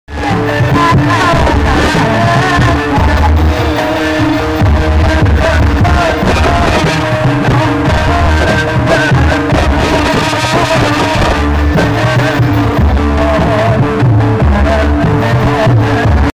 Tamansari Paling Rapi kompak Wahyu manggolo KUDUS
pagelaran seni budaya ketoprak di lapangan pedawang bae kudus jawa tengah